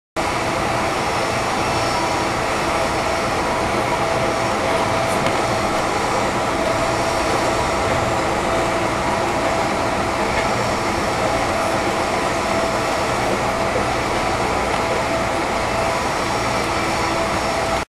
View from the rear end of Amtrak's California Zephyr 2
View from the rear end of Amtrak's California Zephyr in the desert of Utah